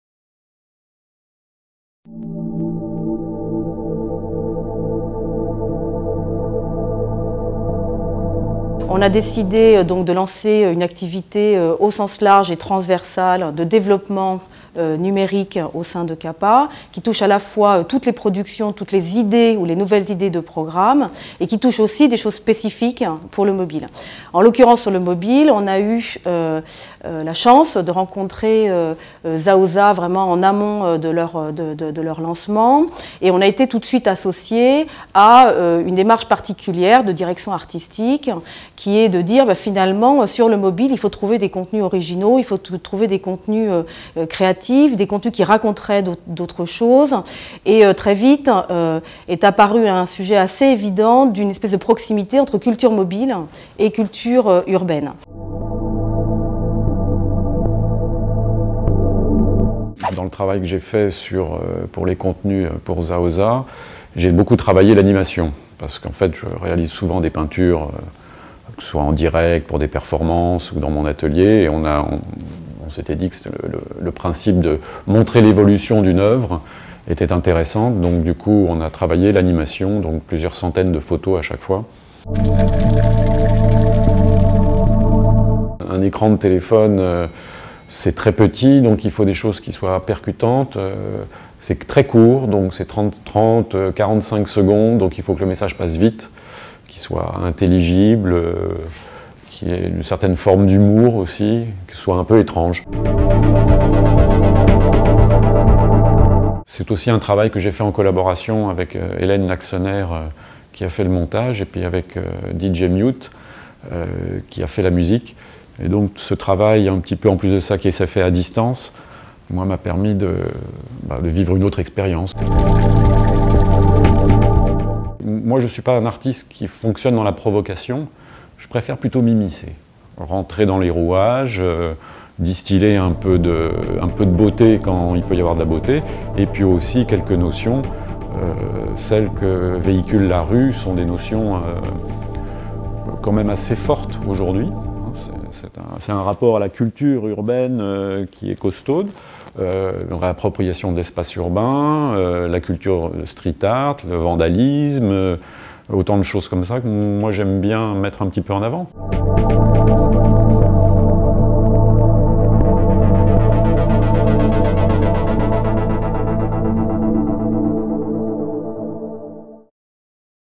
Jean Faucheur, un artiste sur le mobile via Zaoza
En charge des développements numériques du groupe CAPA, Claire LEPROUST a initié un accord avec ZAOZA, le tout nouveau service d'accès à des contenus premium et exclusifs pour le mobile du groupe VIVENDI, dans le but de développer des contenus originaux et forts de sens !

Ainsi, notre première action a été de demander à Jean FAUCHEUR, l'un des premiers artistes urbain, de déporter sa peinture sur le petit écran du mobile.
Réalisée, comme par magie, à la bombe aérosol, une collection d'images et de vidéos en série limitée dévoile alors une beauté et une émotion qui ont déjà séduit plus de 15 000 abonnés au service ZAOZA.